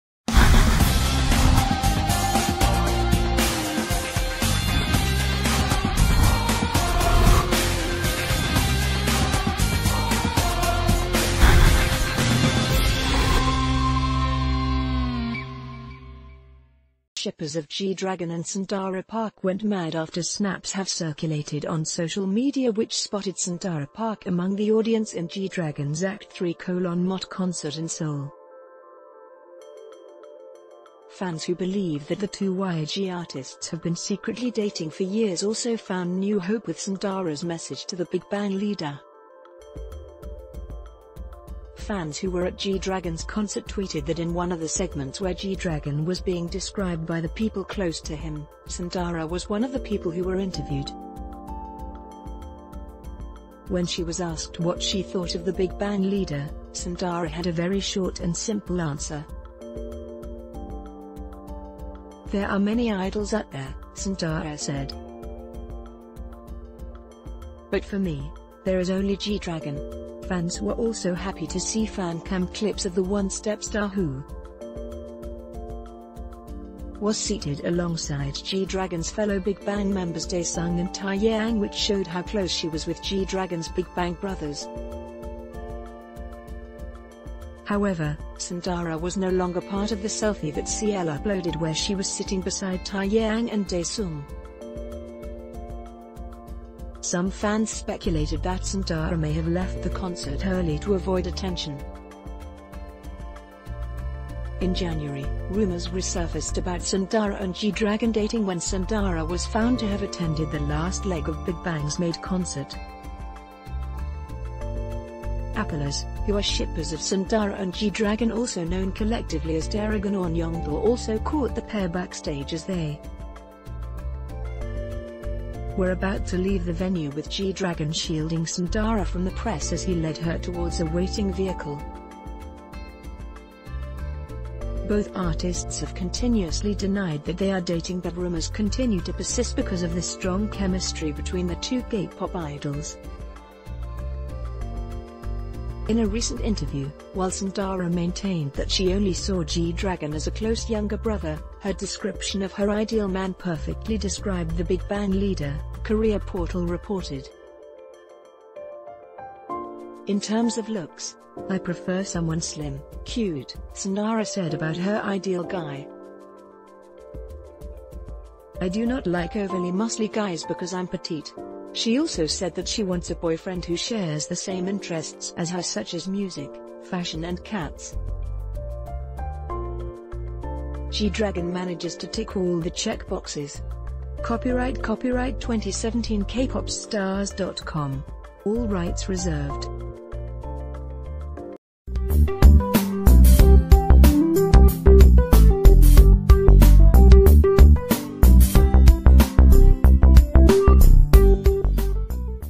G-Dragon & Sandara Park Dating: What Was Sandara's Sweet Message To Rumored Boyfriend At His Conc...
G-Dragon & Sandara Park Dating: What Was Sandara's Sweet Message To Rumored Boyfriend At His Concert?
Shippers of G-Dragon and Sandara Park went mad after snaps have circulated on social media which spo...